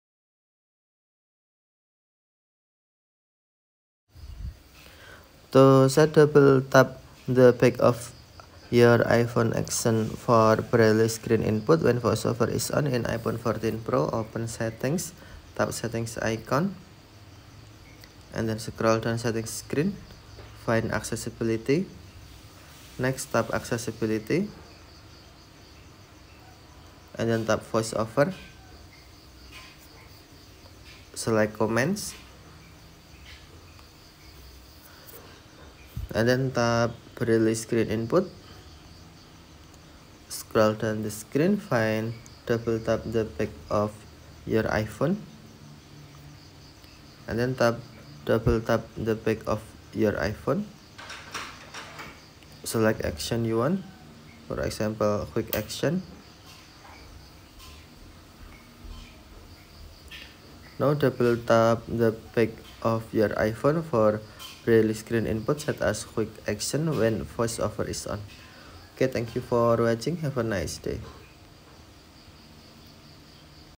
How to set double tap the back of iPhone action for Braille Screen input when voice over is on
On this video you can know How to set double tap the back of iPhone action for Braille Screen input  when voice over is on in iPhone 14 Pro with iOS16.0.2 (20A380) operating system.